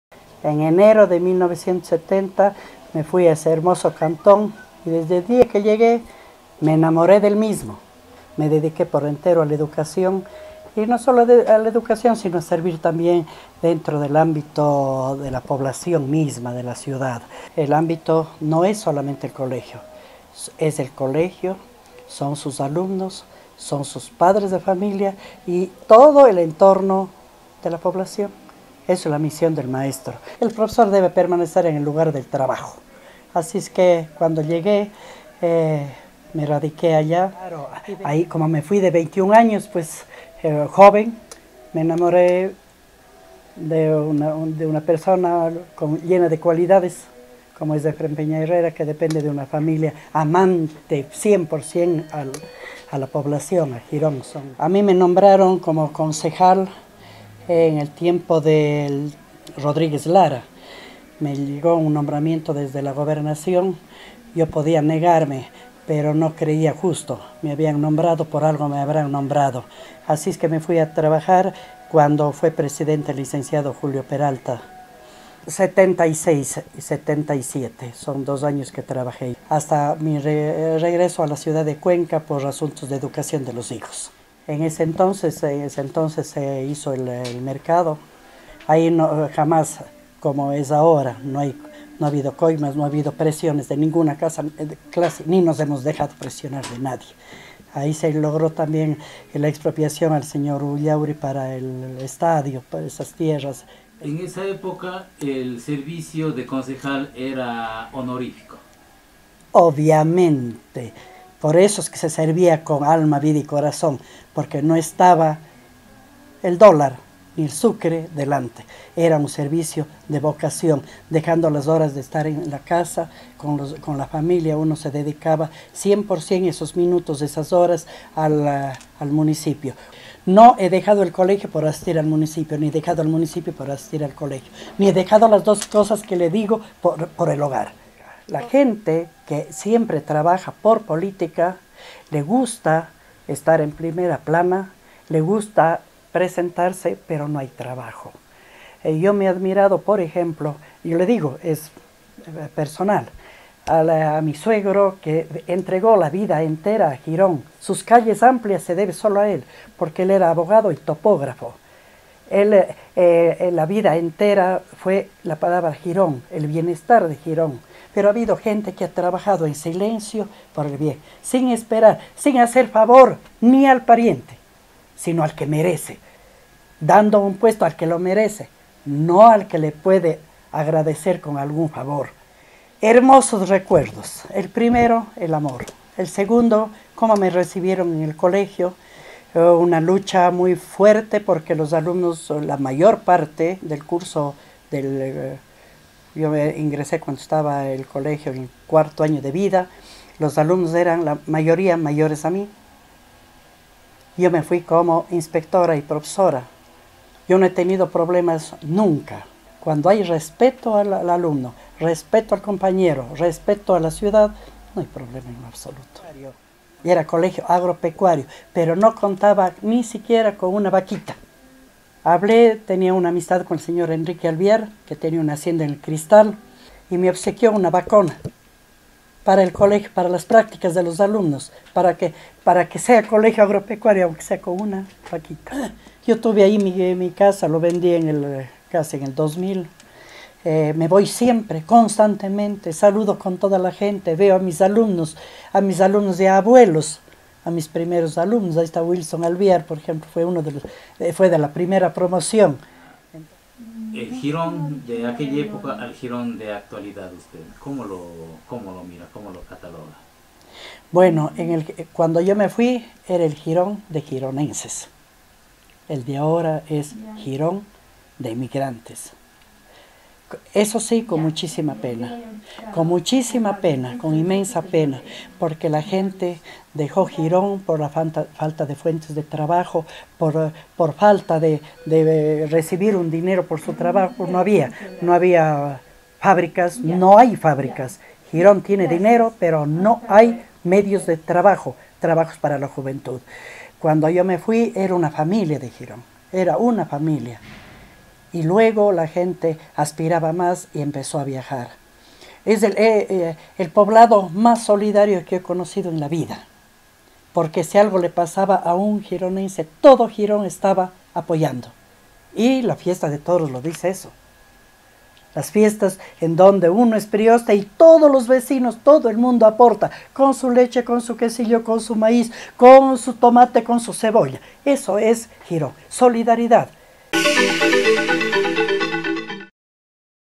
Hace 40 años ejerció funciones la primera mujer concejal de Girón
Martha León Wilches de Peñaherrera, llegó a Girón en enero de 1970 en calidad de profesora de educación física del naciente colegio Alejandro Andrade Cordero, cinco años mas tarde se convertiría en la primera mujer en ingresar al municipio en calidad de concejal, este medio conversó con la persona en mención y nos comentó su experiencia política, educativa y personal, vivida en el cantón del cual se siente parte.
A los 21 años de edad prestó servicio docente en el colegio Alejandro Andrade Cordero.